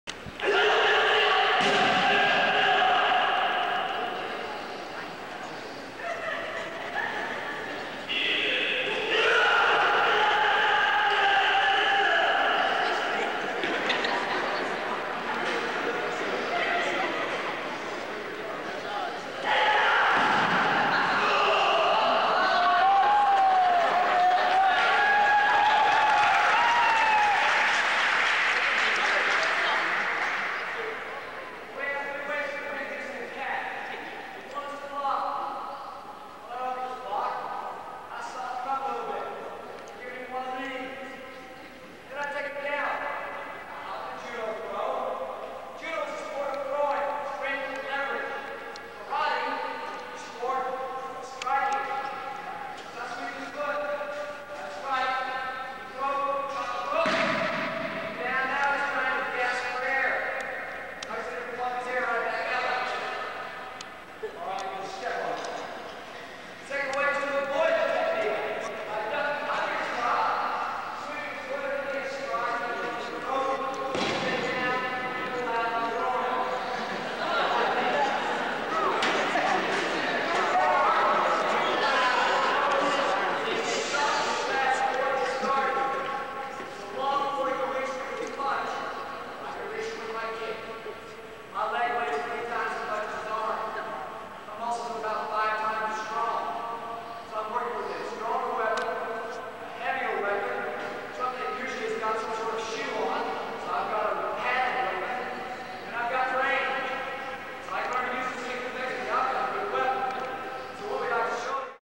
John Graden and Walt Bone Open the Karate Demo at Pinellas Park High School in 1980
Punch, Chop, Punch was always a good open because is was fast and dynamic. Mind you, what is being taught is rubbish, but it's good entertainment.

BloodandGutsKarate.com